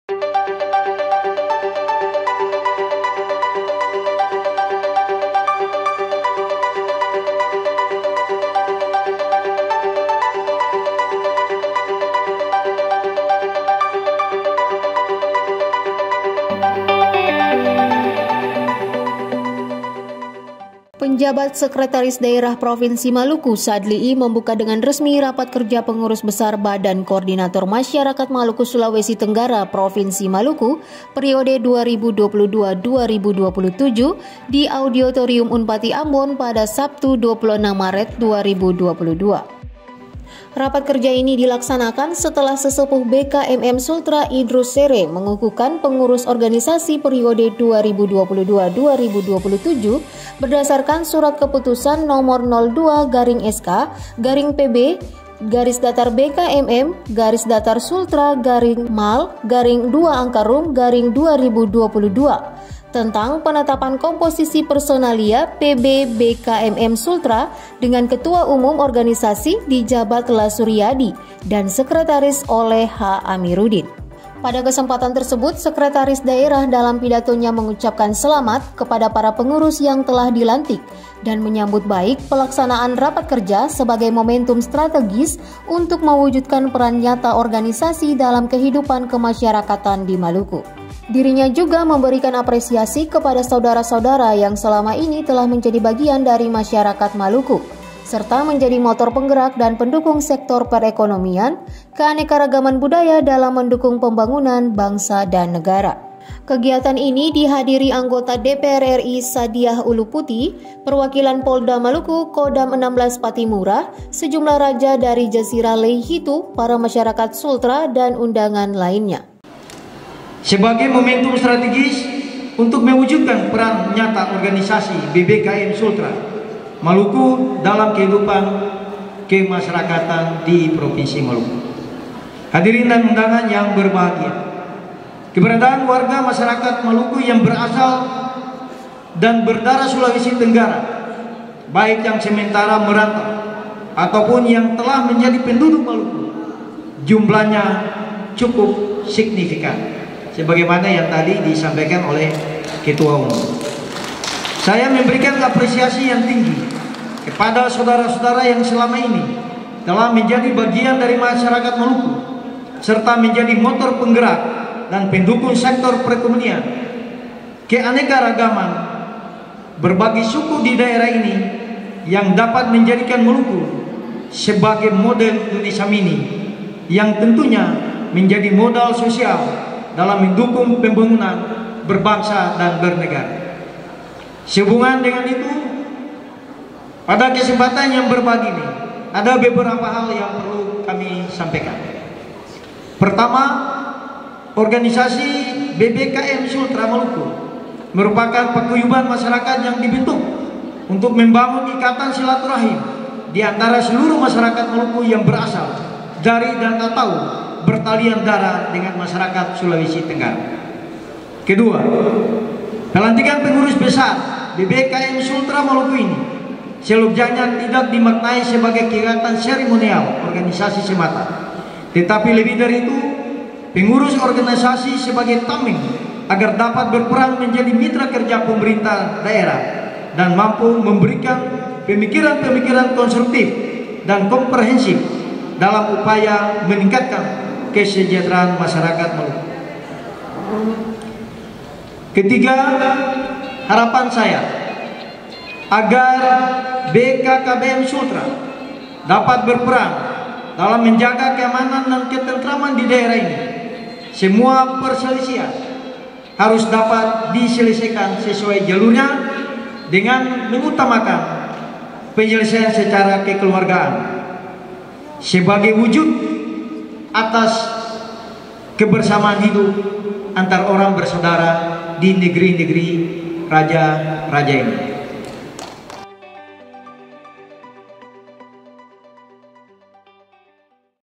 PJ SEKDA MALUKU BUKA RAKER PB BKMM SULAWESI TENGGARA
PADA SABTU (26/3/2022) BERTEMPAT DI AULA LANTAI II GEDUNG REKTORAT UNPATTI AMBON PENJABAT SEKRETARIS DAERAH PROVINSI MALUKU SADALI IE, MELANTIK  DEWAN PENGURUS PUSAT IKATAN MASYARAKAT NELAYAN MALUKU (DPP-IKMANEMA) MASA BAKTI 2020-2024.

KEGIATAN TERSEBUT BERLANGSUNG DI BAWAH SOROTAN TEMA “BERSAMA NELAYAN WUJUDKAN MALUKU MAJU DAN SEJAHTERA”.

ADAPUN PELANTIKAN DILAKUKAN BERDASARKAN SK NOMOR : 001/I.01/KPTS/DP-IKMANEMA/V/2022, TENTANG PENGESAHAN STRUKTUR KEPENGURUSAN DPP IKMANEMA, MASA BAKTI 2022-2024, YANG MENETAPKAN BURHANUDDIN RUMBOW SEBAGAI KETUA UMUM DAN ONTARIO NAHUSONA SEBAGAI SEKRETARIS JENDERAL.

ATAS NAMA PEMERINTAH PROVINSI MALUKU DALAM SAMBUTANNYA, SADALI IE MENGUCAPKAN SELAMAT DAN MENGAPRESIASI PENGURUS IKMANEMA YANG BARU SAJA DILANTIK. MOMENTUM PELANTIKAN INI MERUPAKAN PENGHARGAAN DAN KEPERCAYAAN, SEKALIGUS AMANAH YANG HARUS DIJUNJUNG DENGAN PENUH RASA TANGGUNG JAWAB SEBAGAI BENTUK KEPEDULIAN TERHADAP PENGEMBANGAN POTENSI KELAUTAN DAN PERIKANAN DI MALUKU.

SEHUBUNGAN DENGAN PELANTIKAN, SADALI MENYAMPAIKAN BEBERAPA HAL, SALAH SATUNYA IALAH BAGI MASYARAKAT MALUKU, LAUT MERUPAKAN PRIMADONA UNTUK PEMBERDAYAAN MASYARAKAT MAUPUN PENINGKATAN PENDAPATAN DAERAH, KINI DAN DIMASA YANG AKAN DATANG. UNTUK ITU SADALI MENGAJAK IKMANEMA BERSAMA PEMERINTAH, UNTUK MENJAGA, MENGELOLA DAN MEMANFAATKAN SUMBER DAYA KELAUTAN DAN PERIKANAN BAGI PENINGKATAN KESEJAHTERAAN DAN PEMBANGUNAN DAERAH.